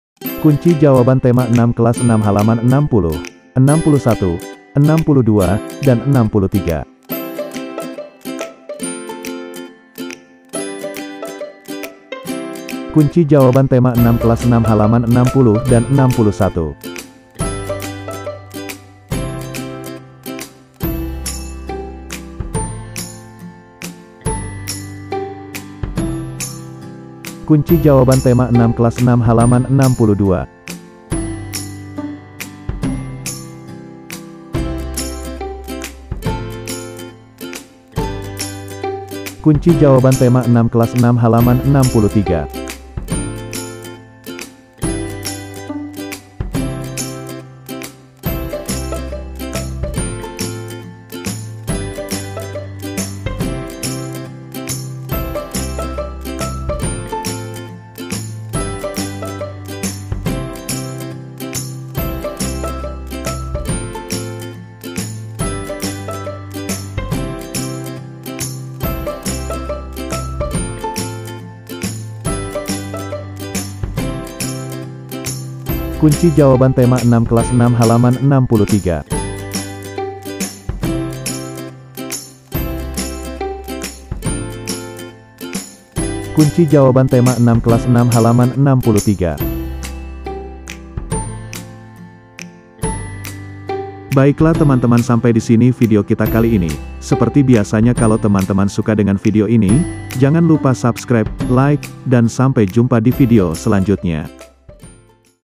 Kunci Jawaban Tema 6 Kelas 6 Halaman 60 61 62 63 | Subtema 2 Pembelajaran 3
Yang mau traktir kopi untuk mendukung channel ini, silahkan klik link dibawah ini, trim’s
00:00 Judul
00:13 Jawaban Tema 6 Kelas 6 Halaman 60 dan 61
00:27 Jawaban Tema 6 Kelas 6 Halaman 62
00:39 Jawaban Tema 6 Kelas 6 Halaman 63
01:33 Video Selanjutnya Jawaban Tema 6 Kelas 6 Halaman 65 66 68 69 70
Catatan : Soal ini berupa pertanyaan terbuka yang artinya ada beberapa jawaban tidak terpaku seperti di video
Source :
• Buku Guru SD/MI Kelas VI Tema 6 Menuju Masyarakat Sejahtera Buku Tematik Terpadu Kurikulum 2013 Edisi Revisi 2018. Jakarta: Kementerian Pendidikan dan Kebudayaan.
• Buku Siswa SD/MI Kelas VI Tema 6 Menuju Masyarakat Sejahtera Buku Tematik Terpadu Kurikulum 2013 Edisi Revisi 2018. Jakarta: Kementerian Pendidikan dan Kebudayaan.
• Berbagai sumber lainnya